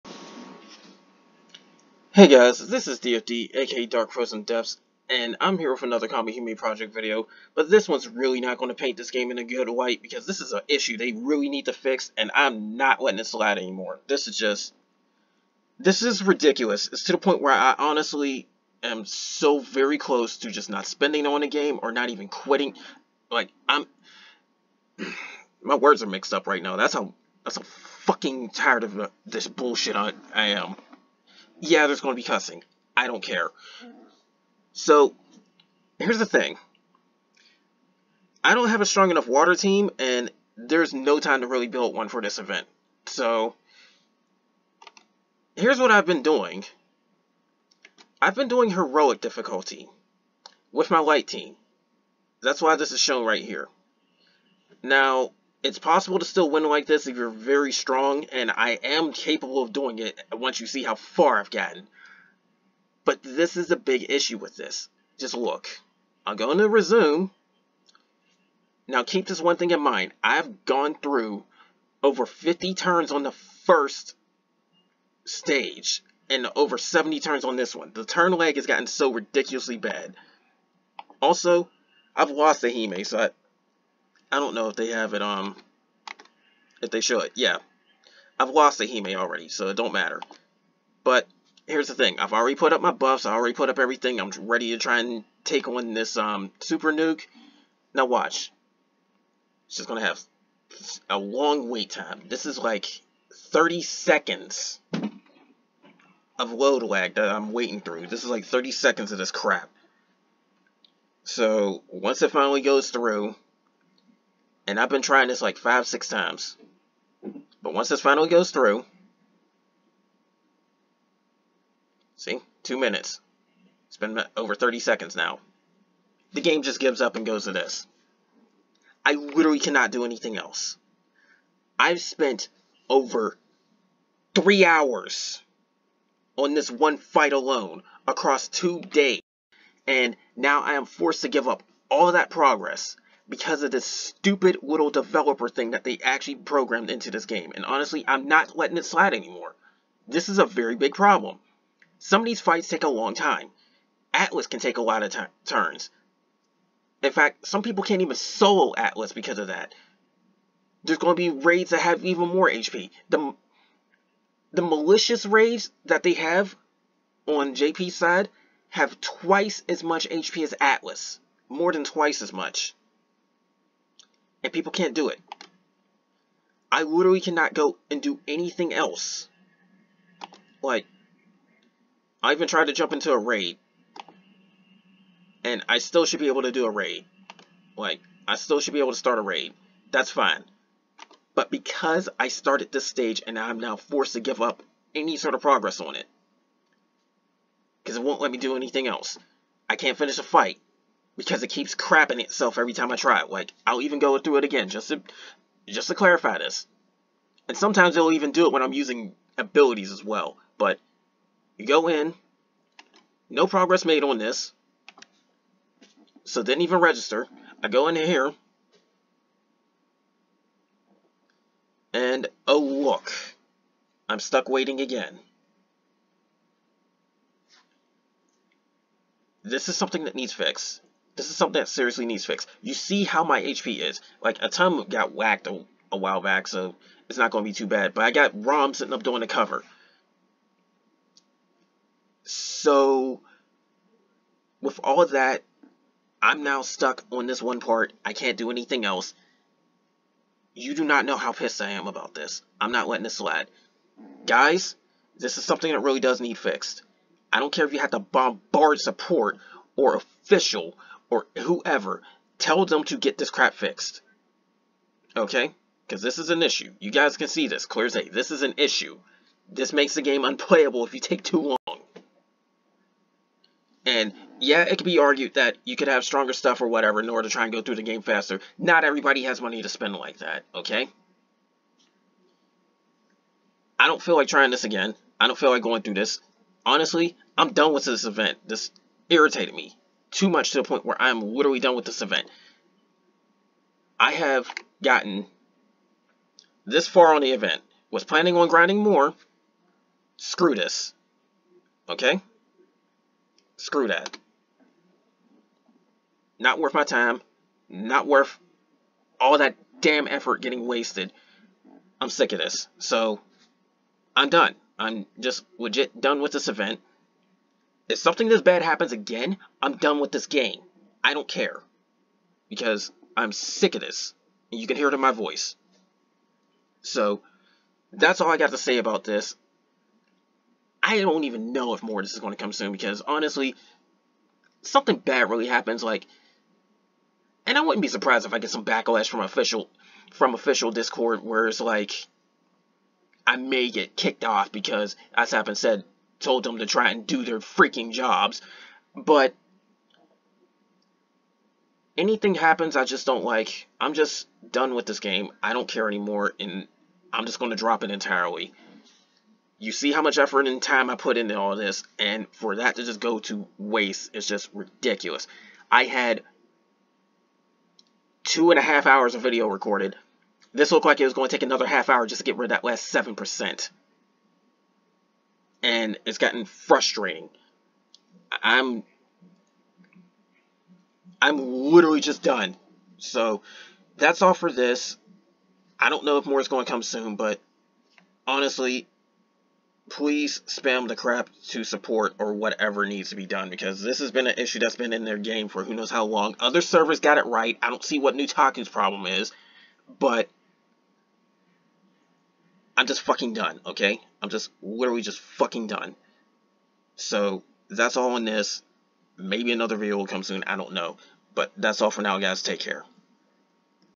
Kamihime PROJECT R - Nutaku Server has a MAJOR Issue
This is something that messed up the game for so long, but now it's gotten so bad...